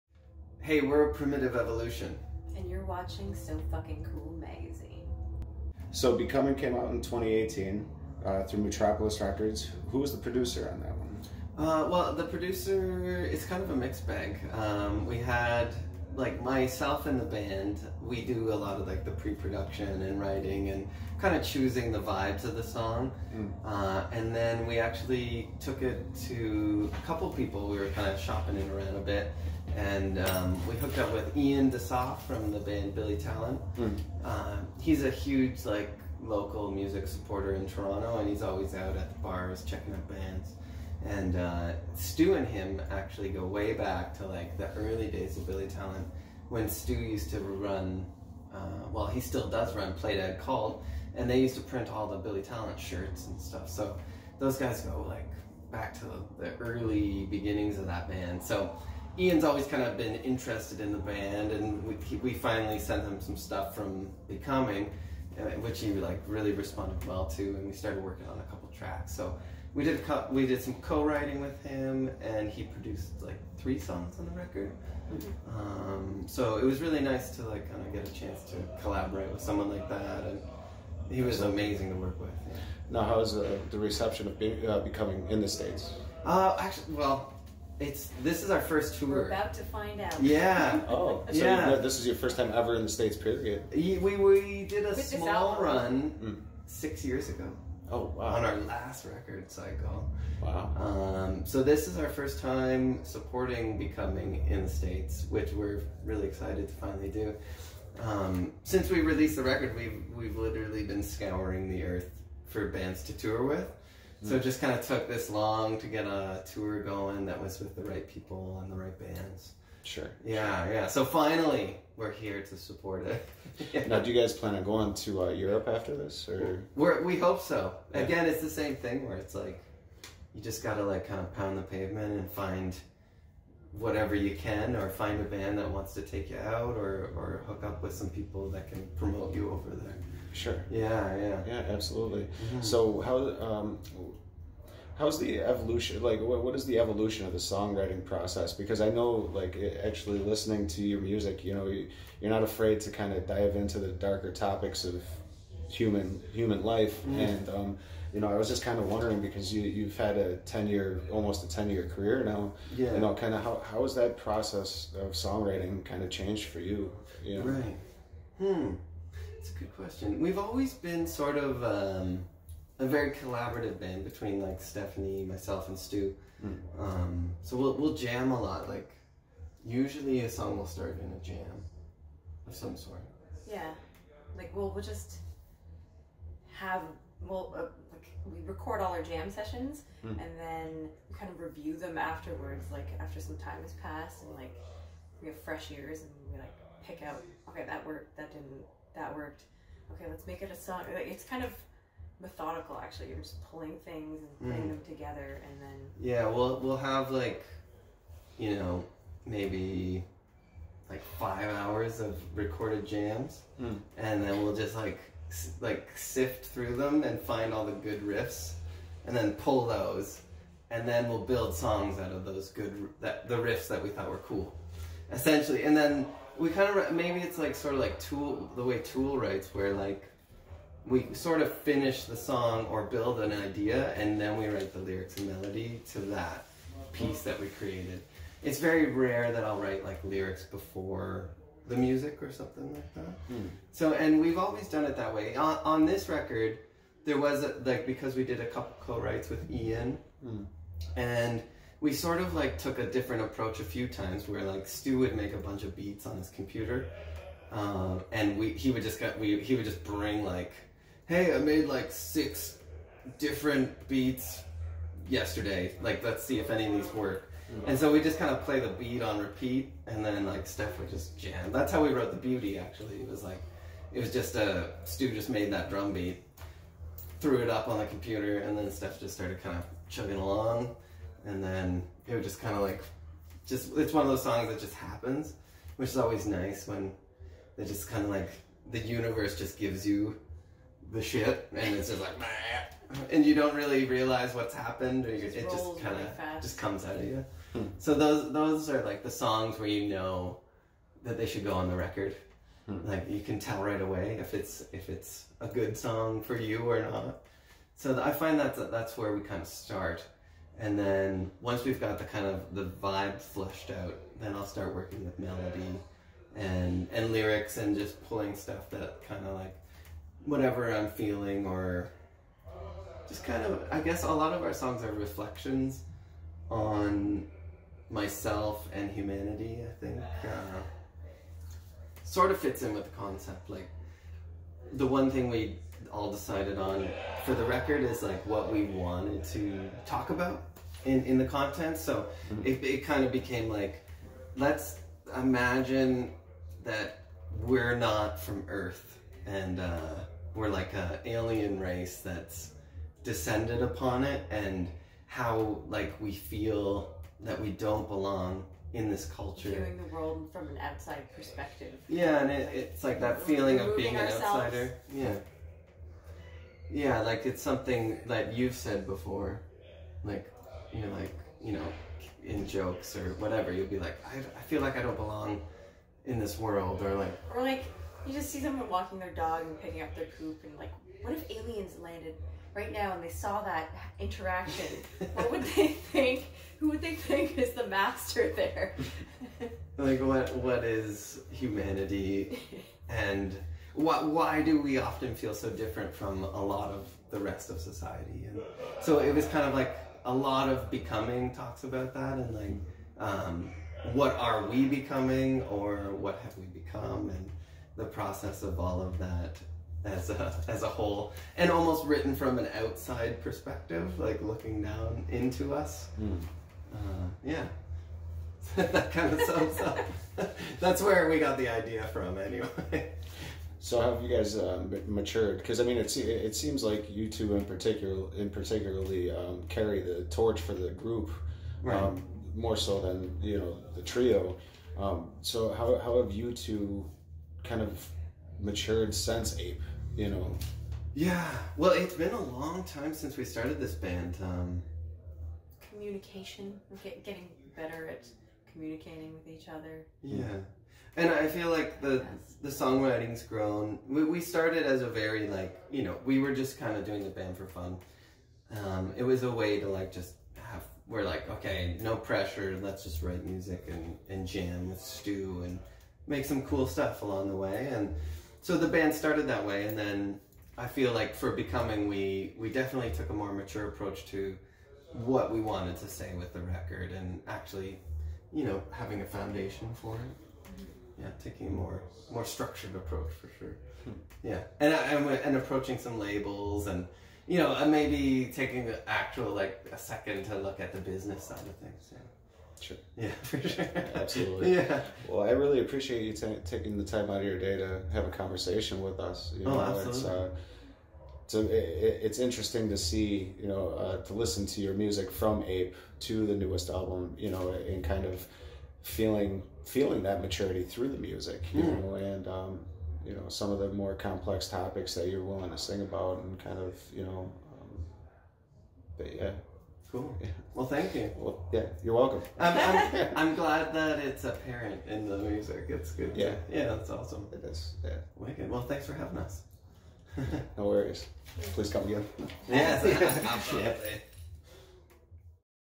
Interview with Primitive Evolution
Sofa King Cool caught up with Primitive Evolution and we talked about the album and tour